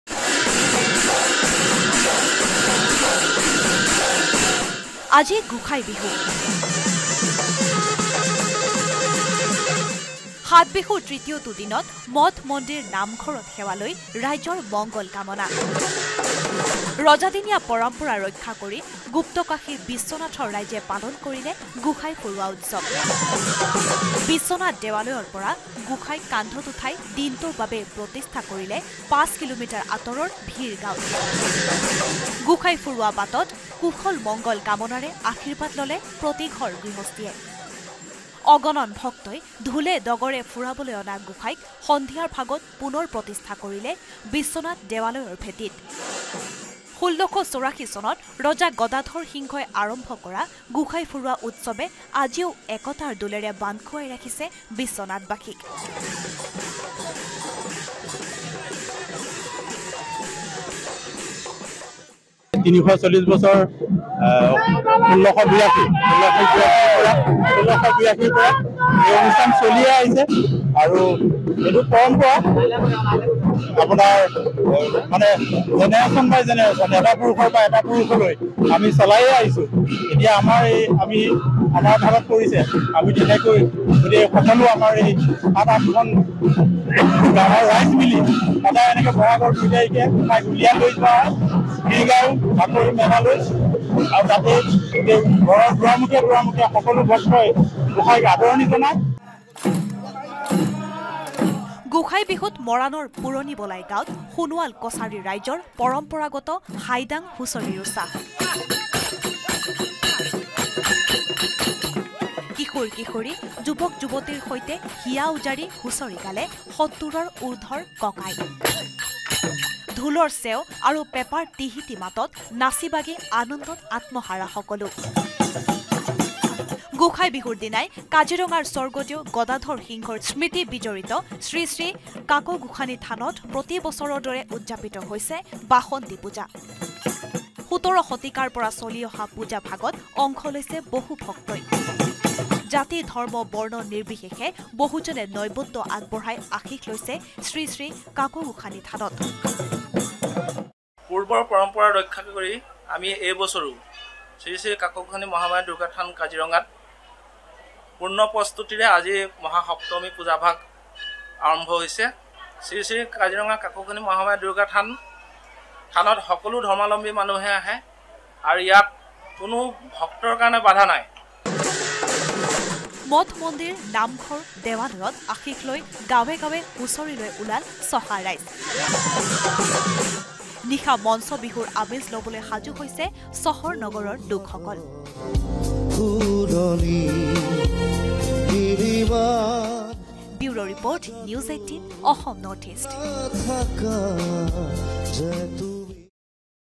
Bohag bihu 2024 | ৰাইজে পালন কৰিলে গোঁসাই ফুৰুৱা উৎসৱ। N18V
Bohag bihu 2024 |  আজি গোসাঁই বিহু। ৰজাদিনীয়া পৰম্পৰা ৰক্ষা কৰি গুপ্তকাশী বিশ্বনাথৰ ৰাইজে পালন কৰিলে গোঁসাই ফুৰুৱা উৎসৱ। বিশ্বনাথ দেৱালয়ৰ পৰা গোঁসাইক কান্ধত উঠাই দিনটোৰ বাবে প্ৰতিষ্ঠা কৰিলে ৫ কিলোমিটাৰ আঁতৰৰ ভিৰগাঁৱত।